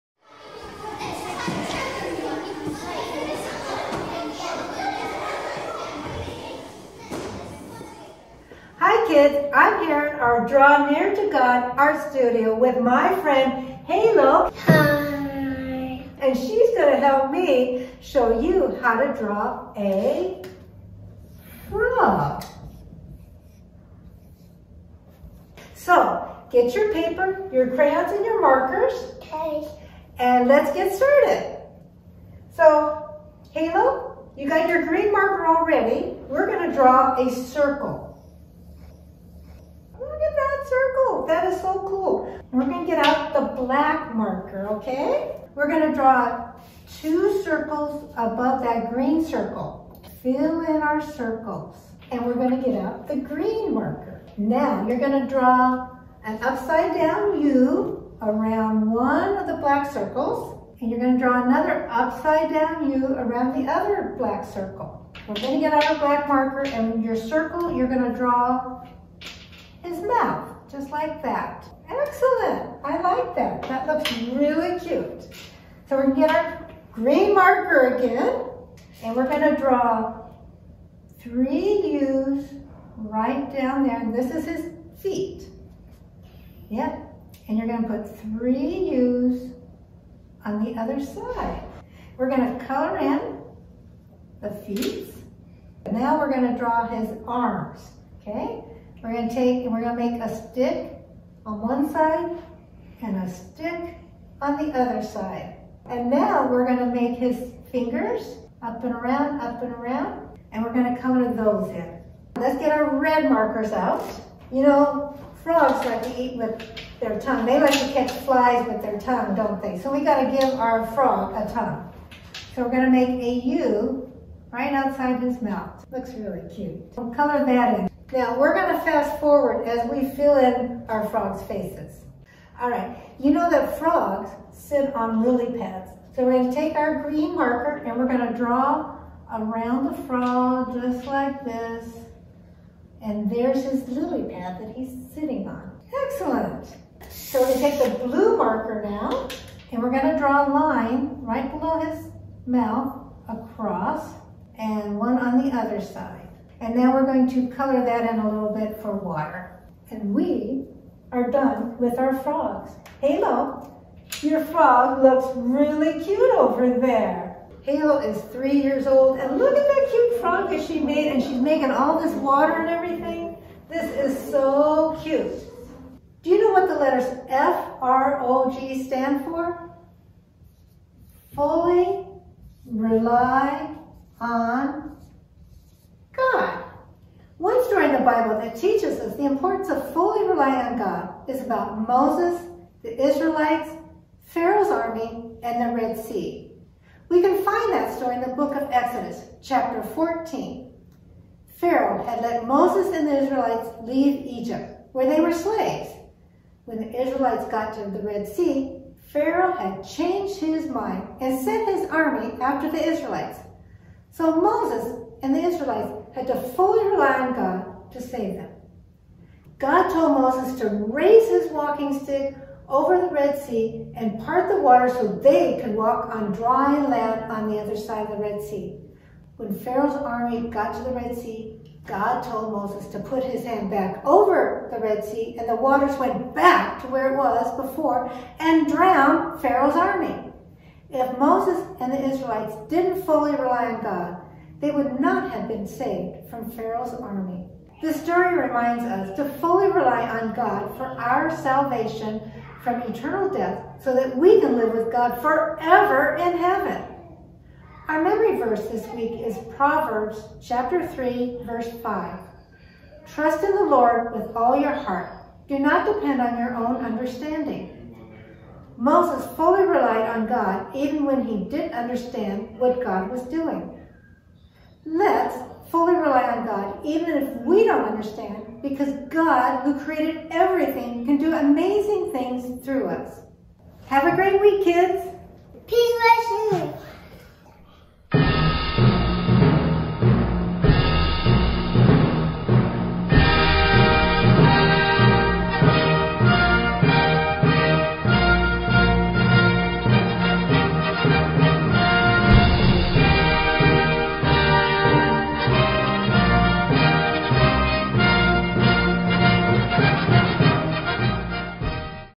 Draw Near To God - Frog Lesson - Proverbs 3:5
Kids, watch Miss Cathy with her friend Halo teach us how to draw a frog, as we learn to
Fully
Rely
On
God.

Memory verse:  Proverbs 3:5
"Trust in the Lord with all your heart.  Do not depend on your own understanding."